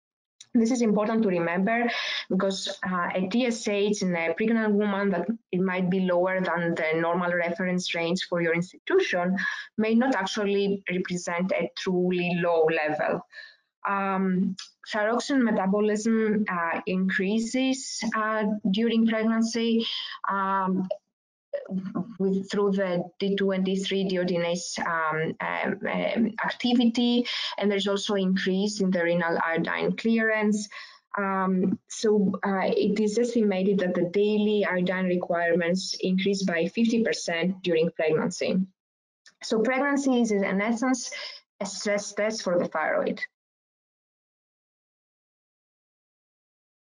Stress Test for the Thyroid
Dr. Spyridoula Maraka explains the natural changes to thyroid activity associated with pregnancy.